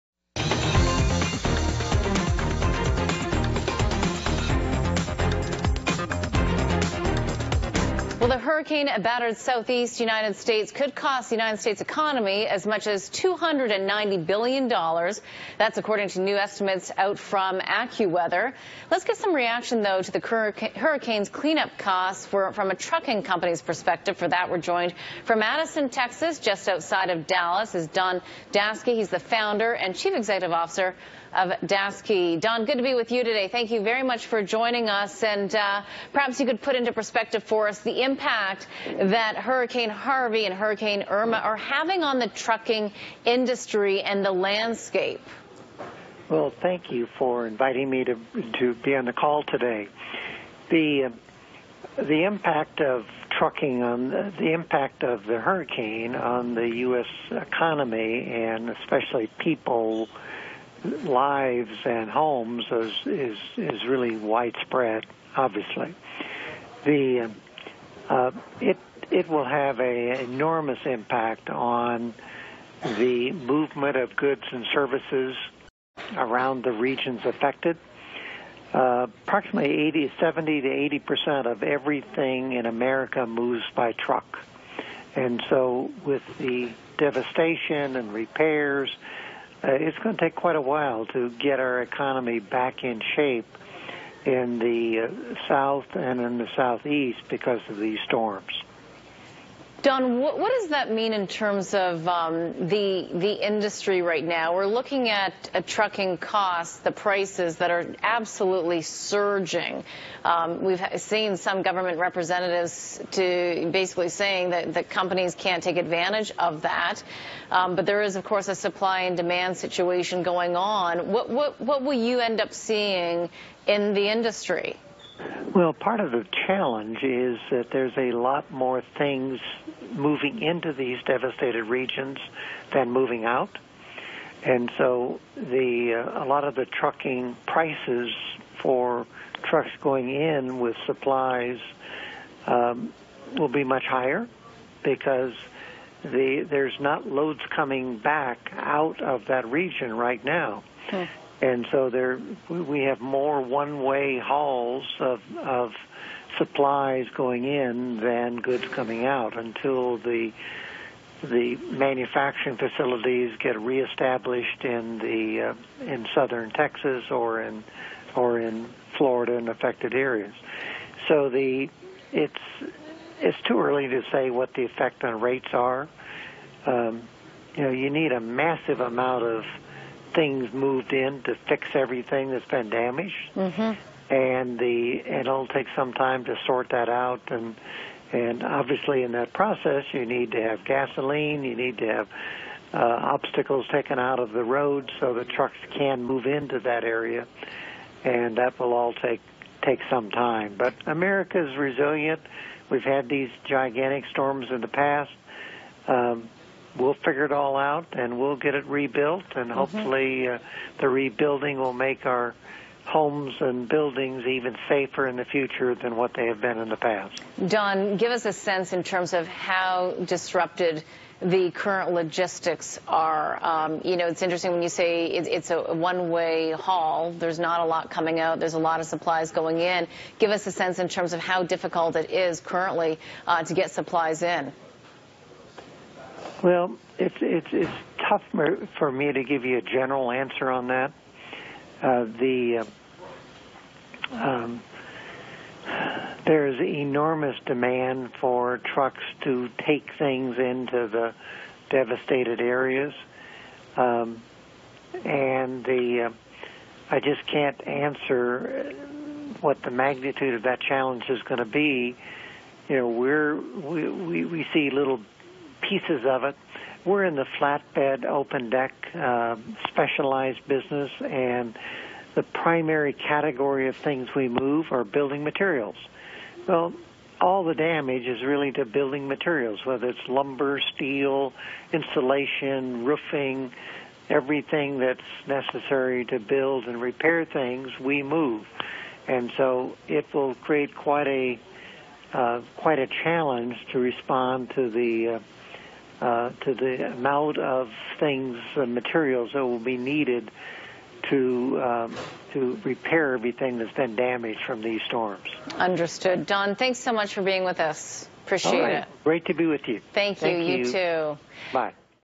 Don Daseke’s (DSKE) Interview with BNN (Canada’s CNN) – Hurricane Damage and Recovery
Don Daseke joins BNN for an interview about the damage caused by Hurricane Harvey and Hurricane Irma and how it will affect the trucking industry.